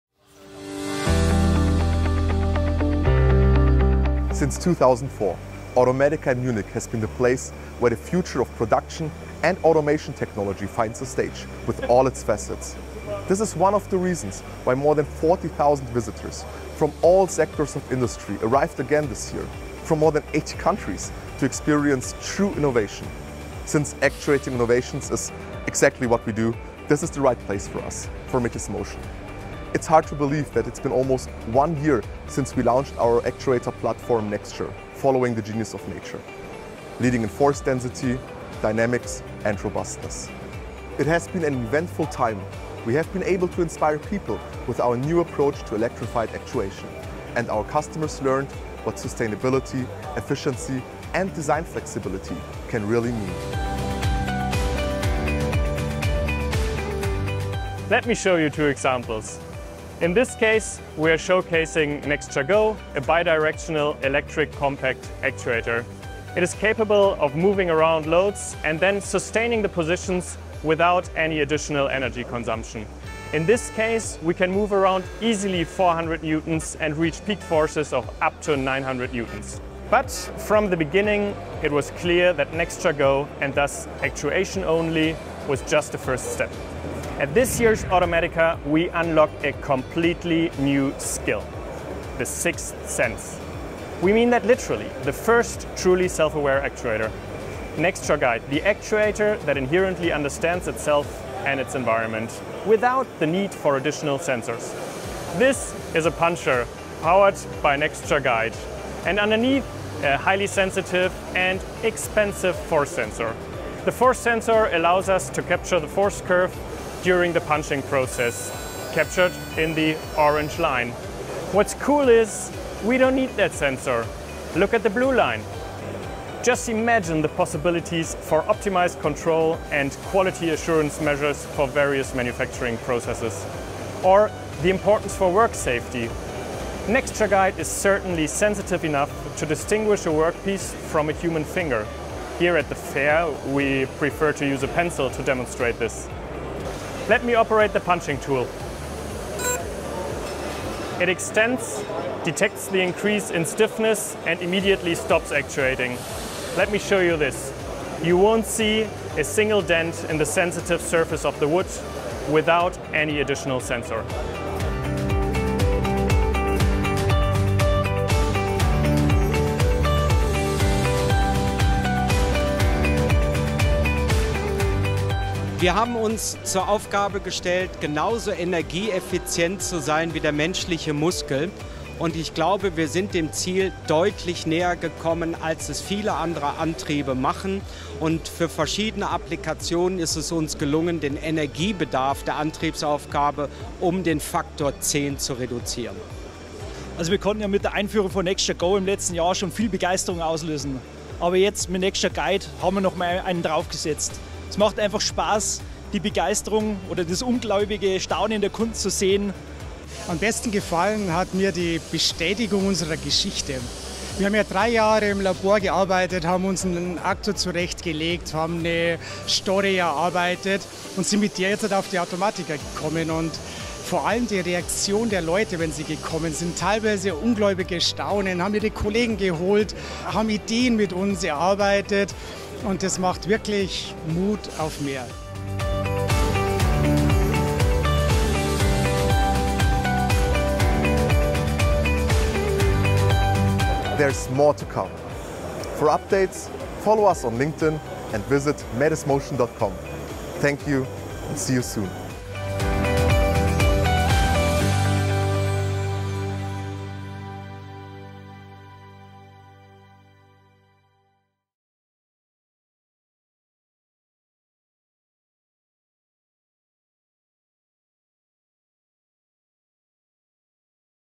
MetisMotion @ automatica 2022 - introducing naXture Guide
Two weeks ago we had two big firsts 🏆:
#1 🎊 MetisMotion made its first trade fair appearance
#2 🧙‍♀️ The world got to see its first actuator with the sixth sense - naXture Guide

Join us and enjoy first-hand impressions from automatica and the launch of naXture Guide - the first actuator that is inherently self-aware of itself and its environment.

From idea, to planning, the big event itself, we could hardly contain our anticipation for what was to come. But your response exceeded our most optimistic hopes.

It becomes more and more clear – it is time to change the world of actuation!

And we will get it done together: with you – colleagues, customers, suppliers and the too often overlooked supporters working in the background.

We are looking forward to more...
...great ideas, emerging when combining diverse fields of expertise,
...meaningful innovation, driven by a true understanding of customer challenges, and
...joy of collaboration, because we are convinced: we succeed together!